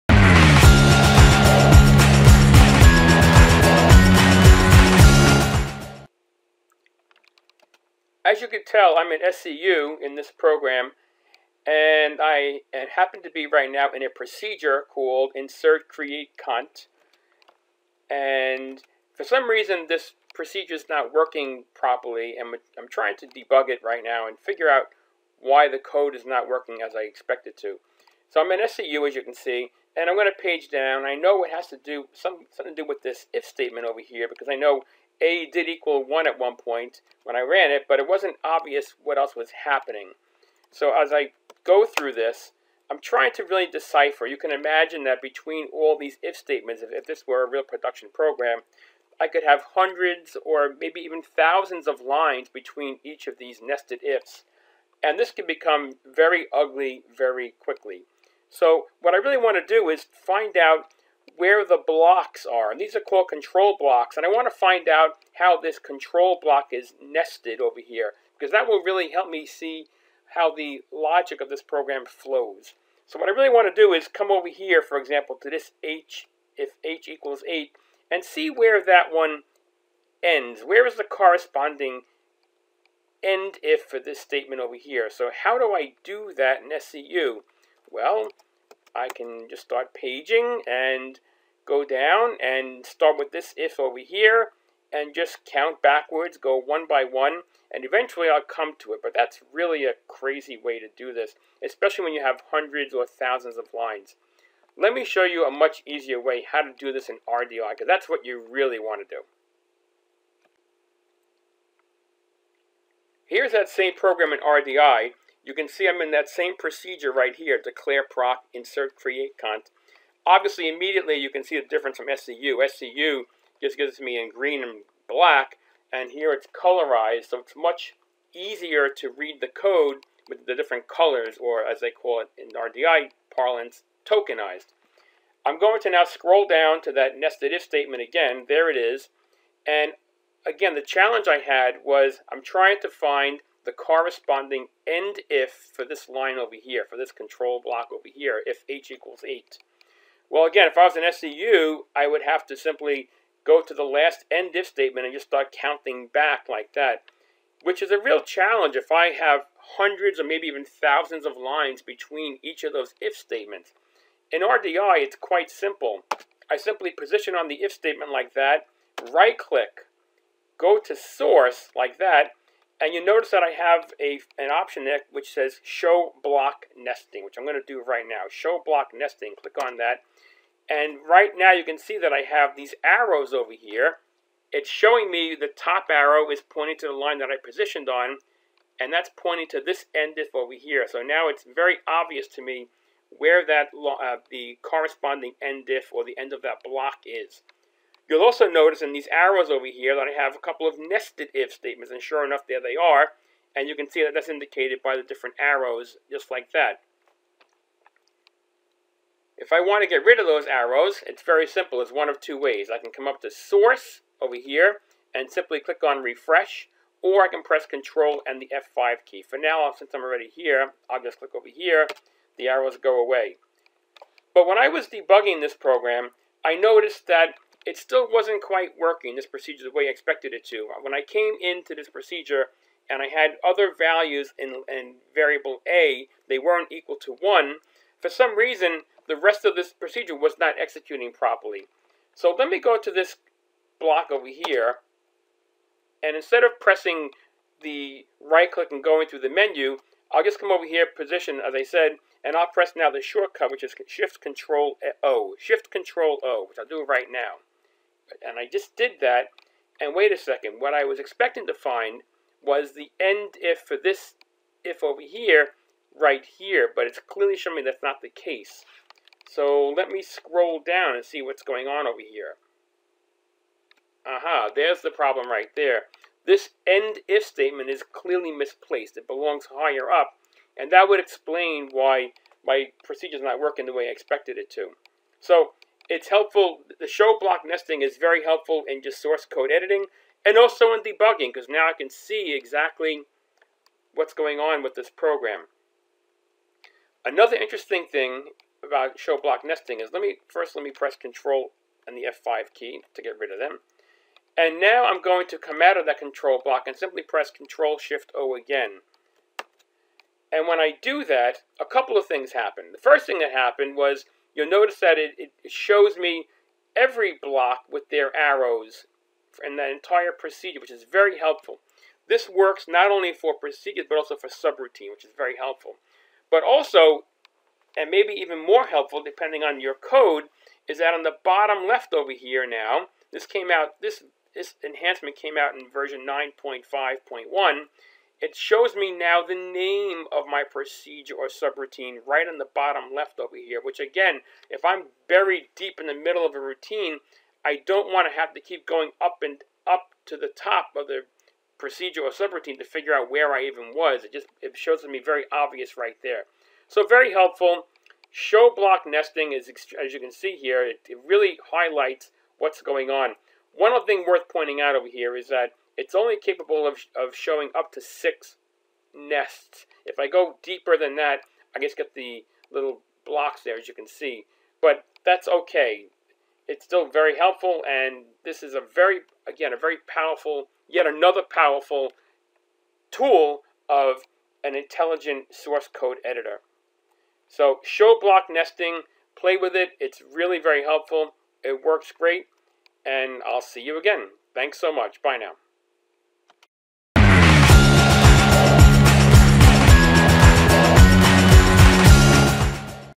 RDi Show Block Nesting in Action
Demonstrates how Show Block Nesting is helpful in both source code maintenance *and* debugging